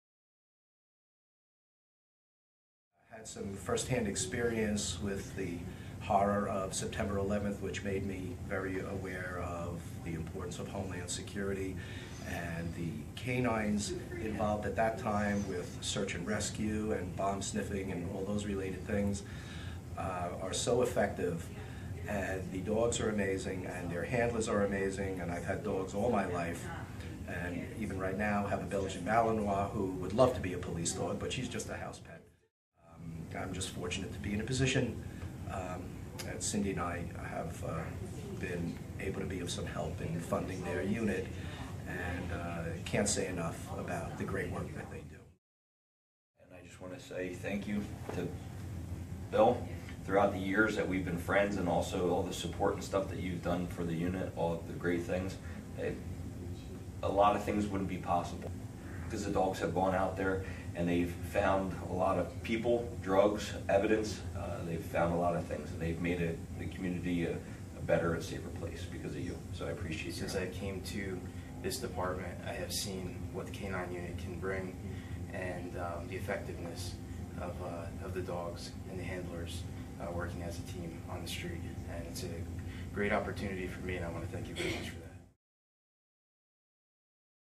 Washington Township police: 'A lot of things wouldn't be possible' without benefactor
Washington Township police officers thank William Peskoff for his donations to the K-9 unit.